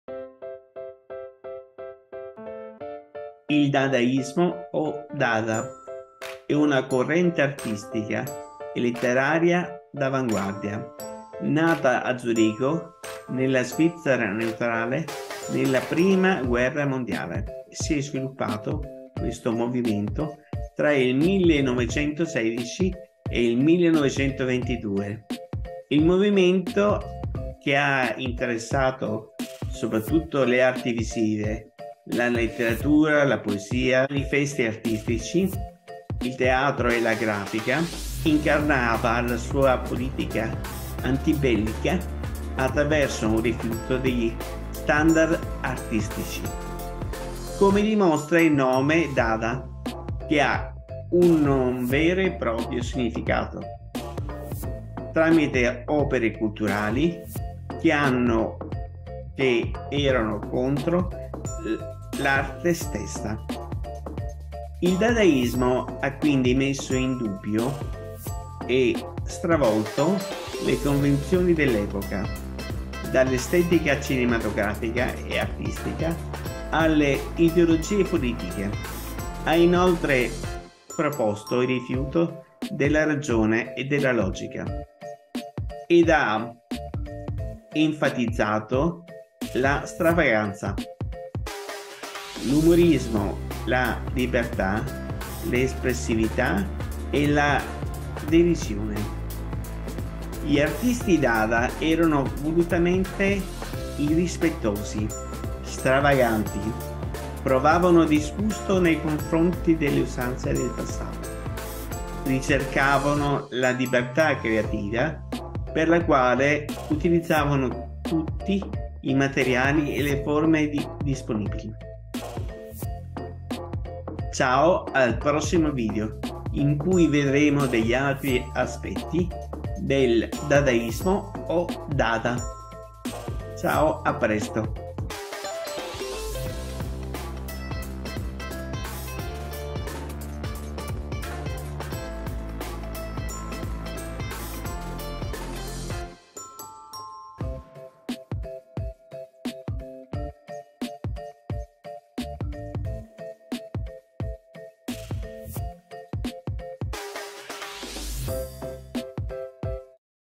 Dadaismo
Il Dadaismo o Dada è una corrente artistica e letteraria d'avanguardia nata a Zurigo, nella Svizzera neutrale nella prima guerra mondiale, e sviluppatasi tra il 1916 e il 1922. Il movimento, che ha interessato soprattutto le arti visive, la letteratura (poesia, manifesti artistici), il teatro e la grafica, incarnava la sua politica antibellica attraverso un rifiuto degli standard artistici, come dimostra il nome dada che non ha un vero e proprio significato, tramite opere culturali che erano contro l'arte stessa.

Il Dadaismo ha quindi messo in dubbio e stravolto le convenzioni dell'epoca, dall'estetica cinematografica e artistica, alle ideologie politiche; ha inoltre proposto il rifiuto della ragione e della logica, ed ha enfatizzato la stravaganza, l'umorismo, la libertà espressiva e la derisione. Gli artisti dada erano volutamente irrispettosi, stravaganti, provavano disgusto nei confronti delle usanze del passato; ricercavano la libertà creativa per la quale utilizzavano tutti i materiali e le forme disponibili.